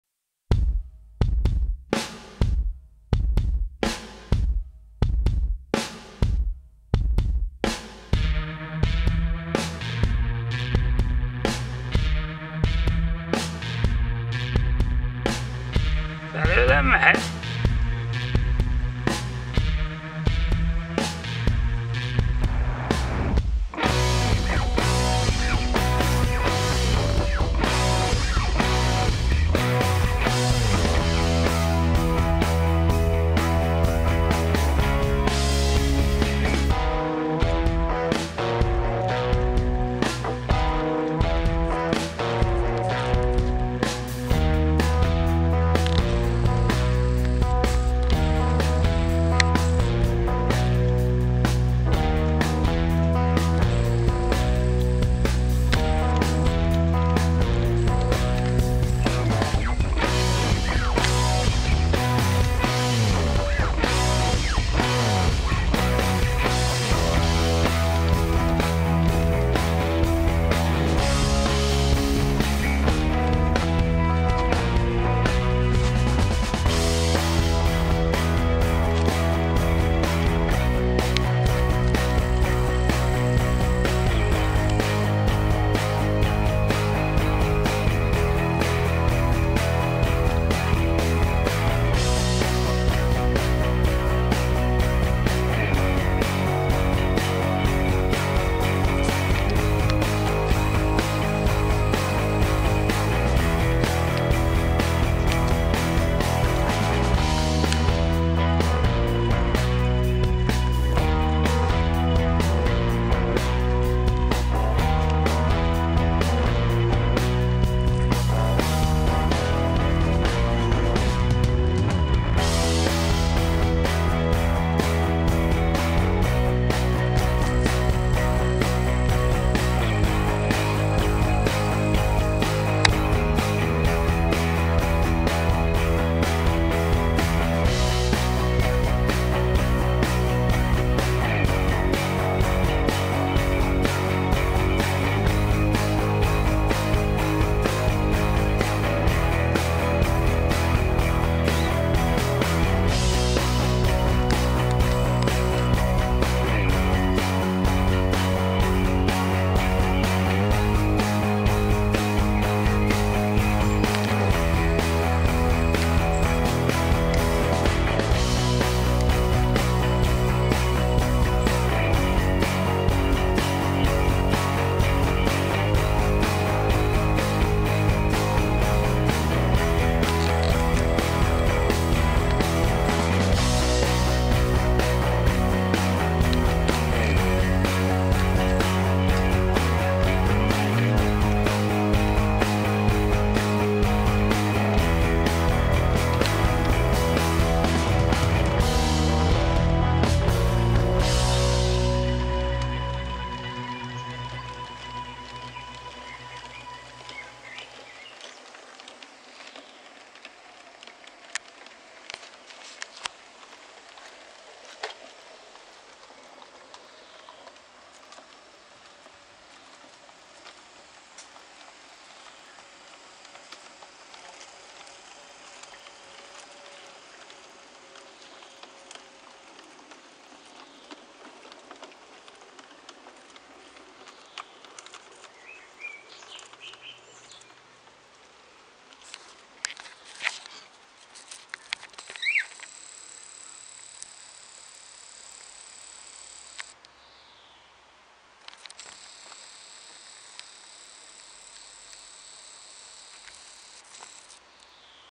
R.K.V.t.
Az én kerti vasutam. Egy kis előfogatolás, csak most normál fényviszonyok között.